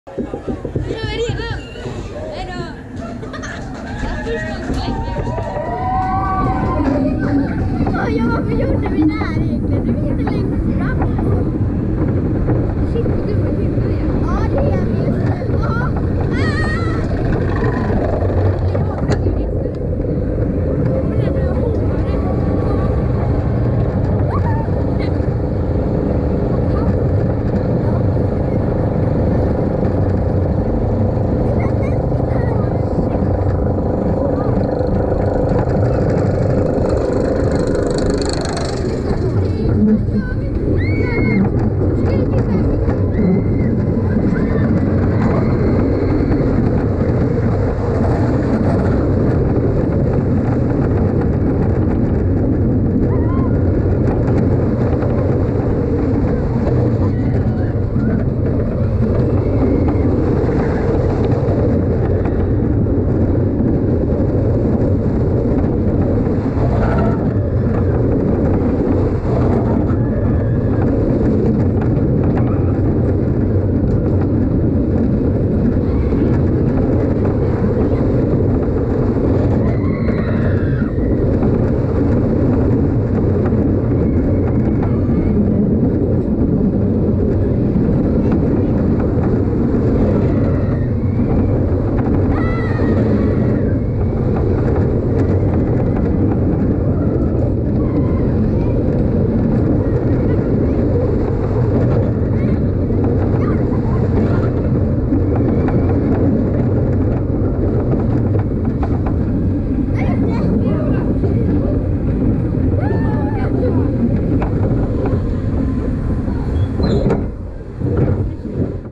Åk Balder – hemifrån! (POV)
Längtar du också efter pirr i magen och snabba åk? Ta ett åk hemma i vardagsrummet i härliga Balder, 100% pirr och glädje! Redan i första backen (med 70° lutning!) börjar åkturen över toppar och dalar. Under tiden upplever du negativa G-krafter hela 10 gånger och det känns som att du lyfter från sätet och nästan flyger.